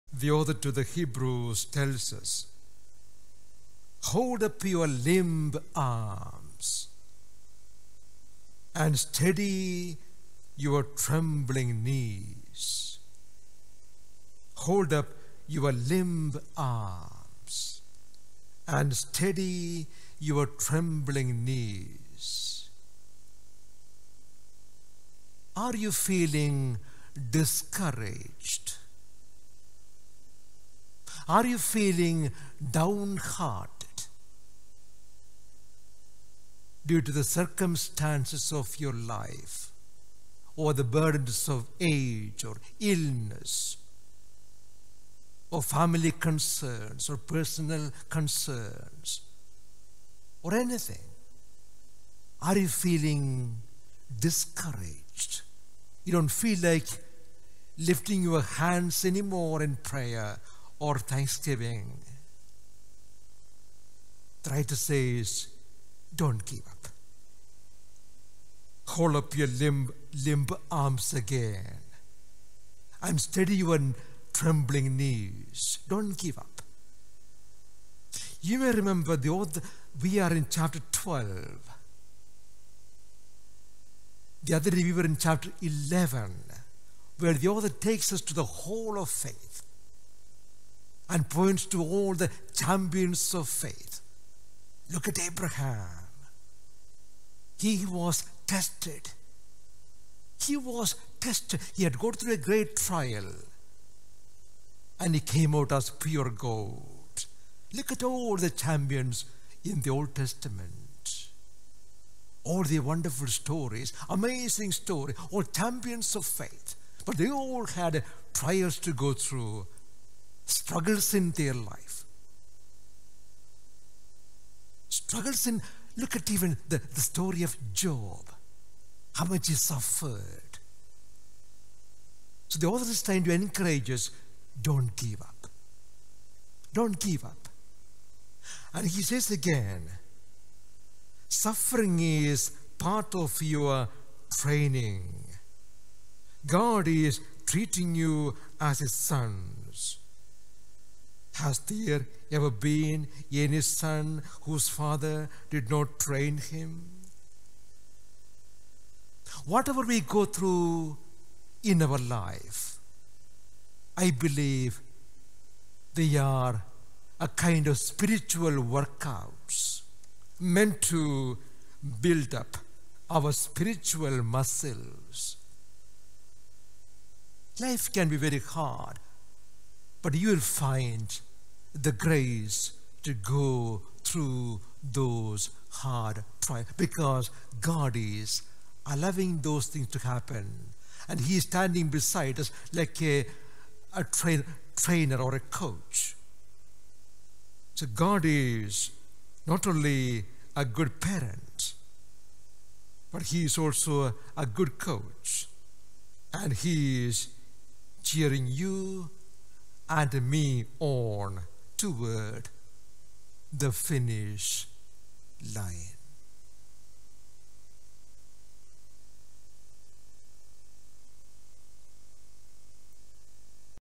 Homily 3rd February 2021 Hebrews 12 :4:15 God a good parent and a good coach.
God our Father is not only a good parent but he is also a good coach and he is cheering you on toward the finish line.